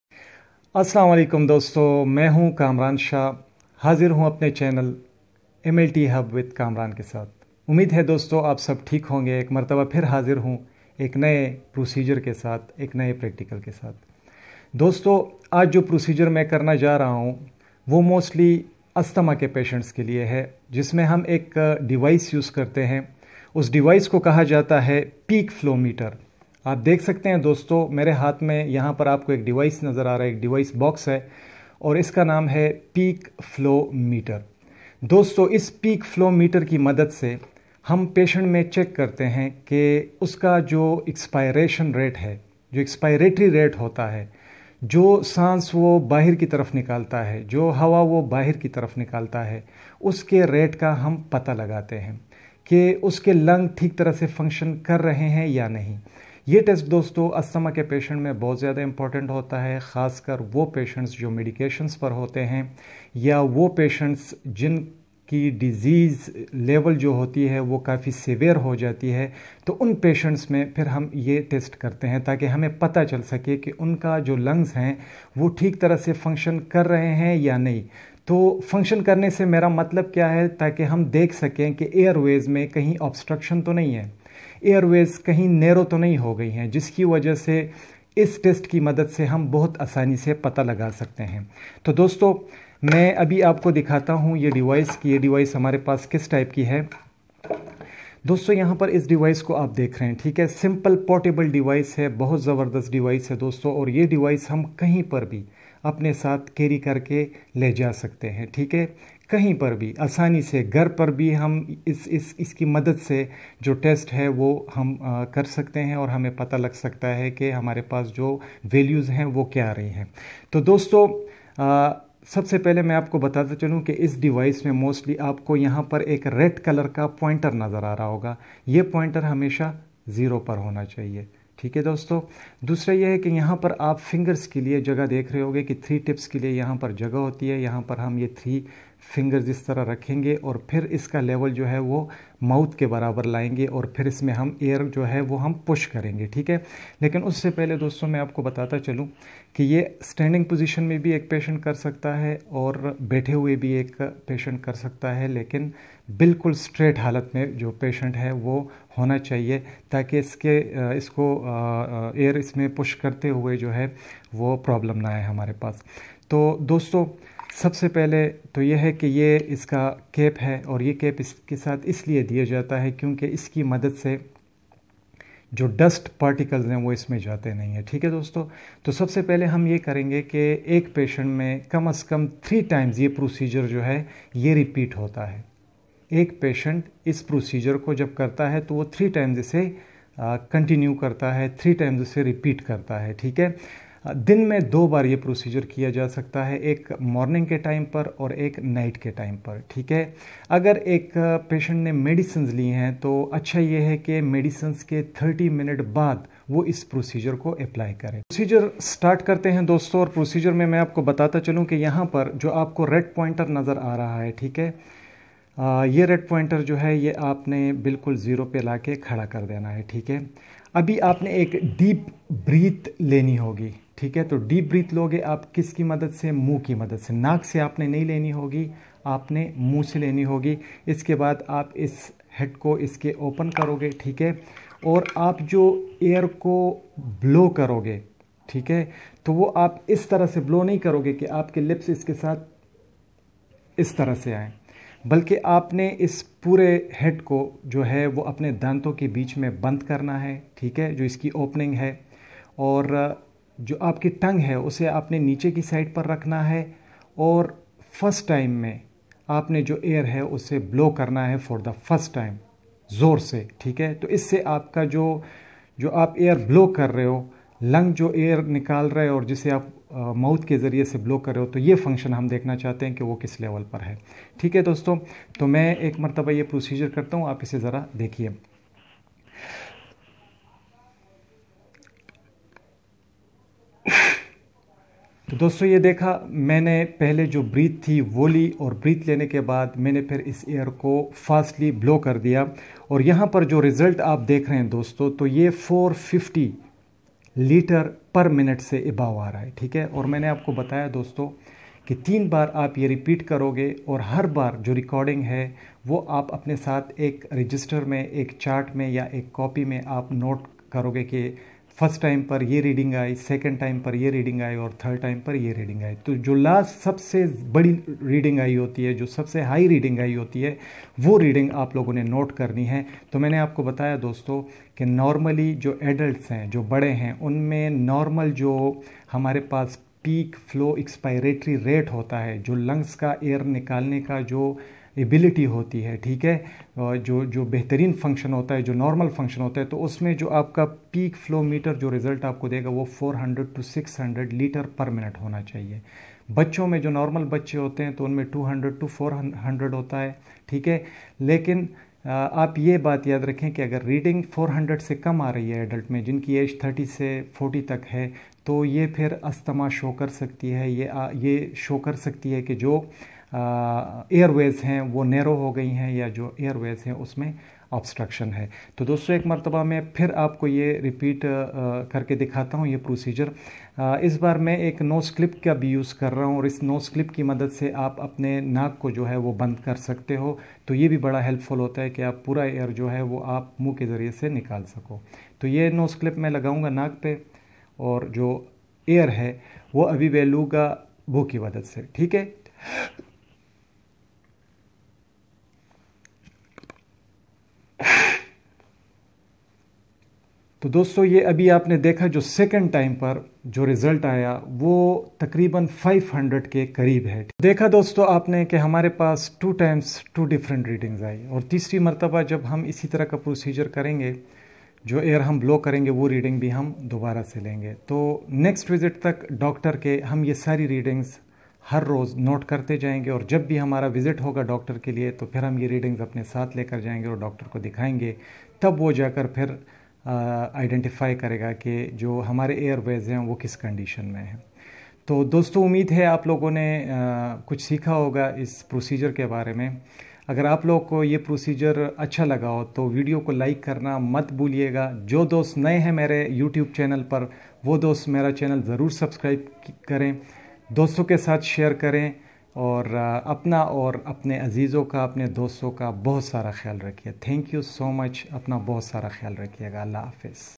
How to use a peak flow meter
Dear friends in this video you will learn how to use peak flow meter, and you will know what is asthma and how you can take readings through peak flow meter and identify the severity of asthma, as well as you will know the fact that this test is useful in identification of airways narrowness and well as obstruction.

Your Queries in the Video:
00:00 | Introduction
01:31 | Peak flow meter device
04:38 | Procedure of Peak flow meter